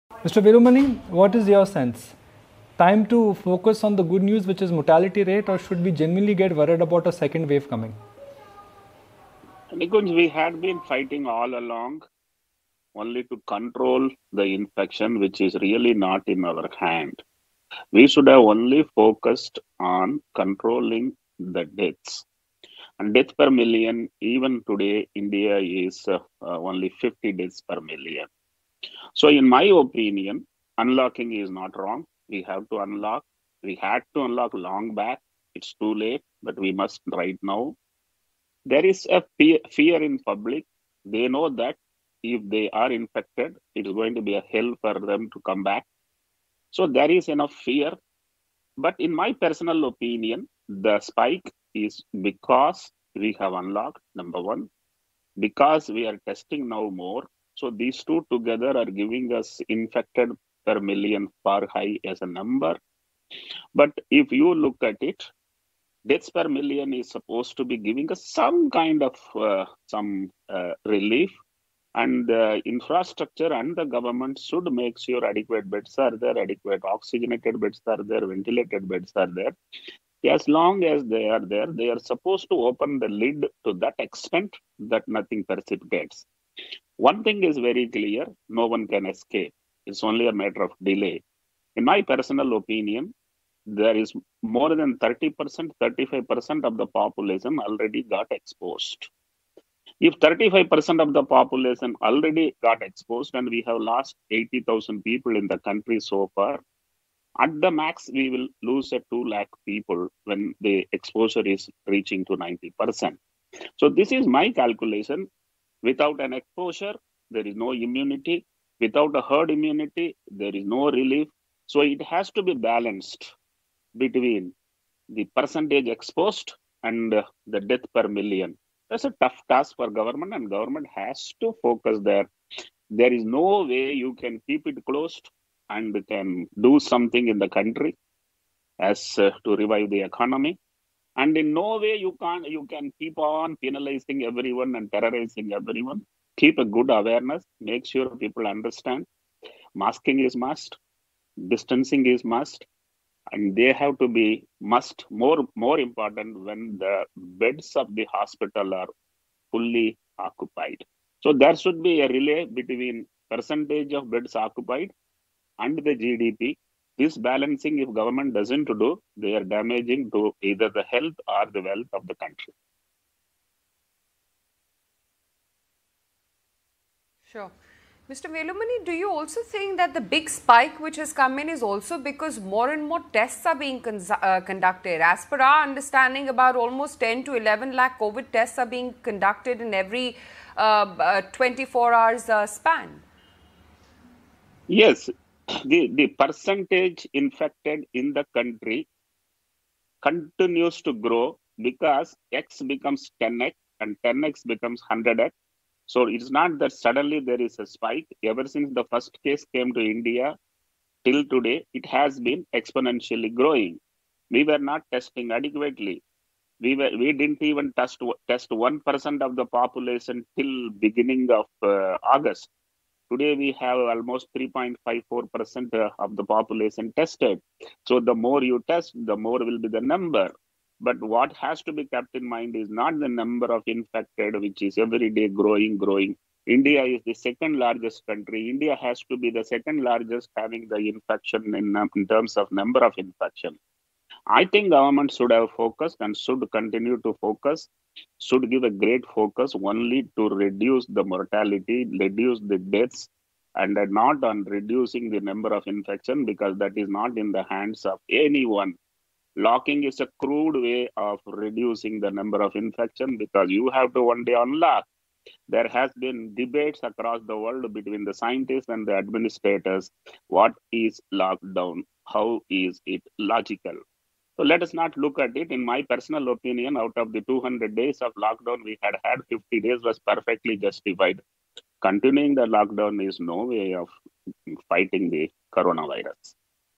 Without herd immunity there is no relief from Covid-19: A Velumani, Thyrocare Tech
Unlocking plus greater testing is leading to spike in Corona positive numbers, but deaths per million are very, very low. Government has to balance halth and wealth of the country, says A Velumani, Chairman, CEO & MD, Thyrocare Tech.

For Markerts udpates and business news on the go, download ETMarketes apps

ET Markets | A Times Internet Limited product